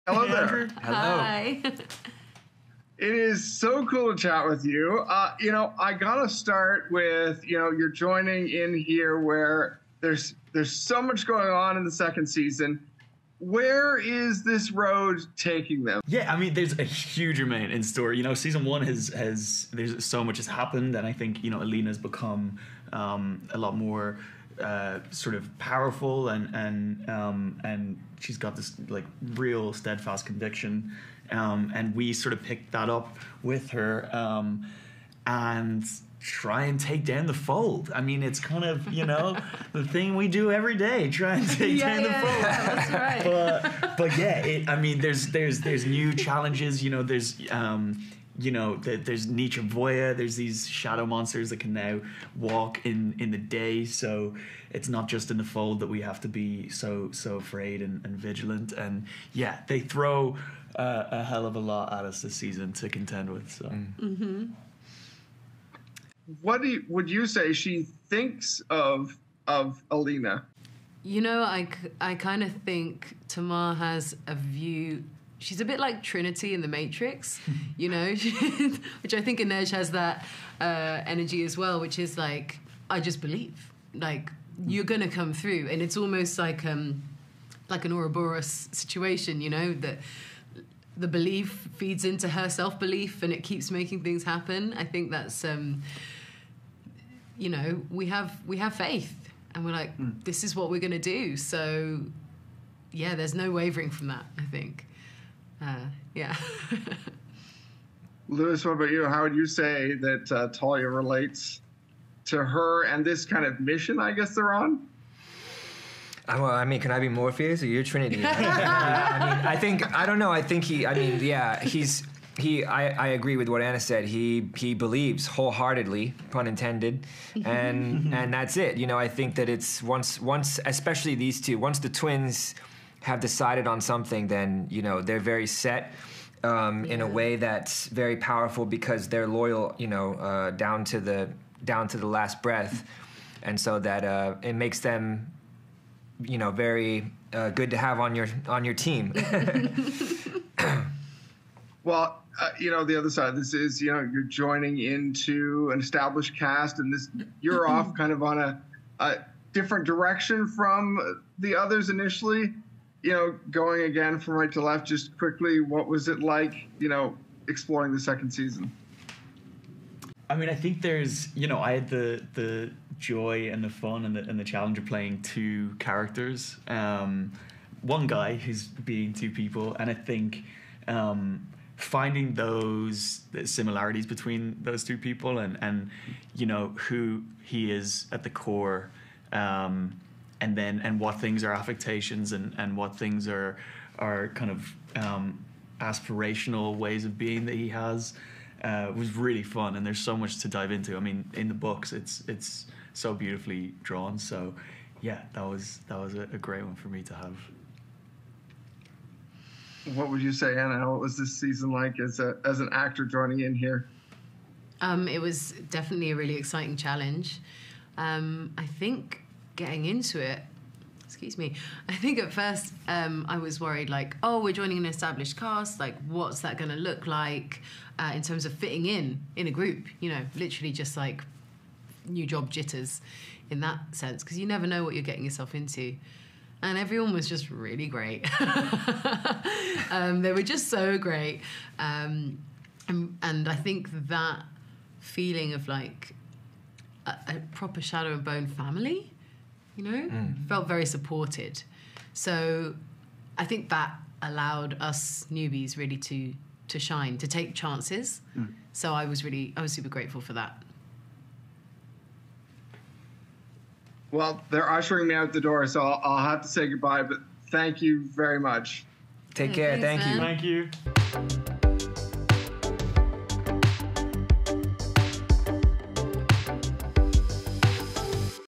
Lewis Tan, Anna Leong Brophy & Patrick Gibson on Shadow and Bone season 2
The GATE sat down with Shadow and Bone's Lewis Tan (Tolya Yul-Bataar), Anna Leong Brophy (Tamar Kir-Bataar) and Patrick Gibson (Nikolai Lantsov) to talk about season 2, and working with this large, ensemble cast.

Synopsis: Alina Starkov is on the run. A beacon of hope to some and a suspected traitor to others, she's determined to bring down the Shadow Fold and save Ravka from ruin. But General Kirigan has returned to finish what he started. Backed by a terrifying new army of seemingly indestructible shadow monsters and fearsome new Grisha recruits, Kirigan is more dangerous than ever. To stand a fighting chance against him, Alina and Mal rally their own powerful new allies and begin a continent-spanning journey to find two mythical creatures that will amplify her powers. Back in Ketterdam, the Crows must forge new alliances as they contend with old rivals and even older grudges that threaten not only their place in the Barrel, but their very lives. When a chance at a deadly heist comes their way, the Crows will once again find themselves on a collision course with the legendary Sun Summoner. Based on Leigh Bardugo's worldwide bestselling Grishaverse novels, SHADOW AND BONE returns for a second season of new friendships, new romance, bigger battles, epic adventures — and a shocking family secret that could shatter everything.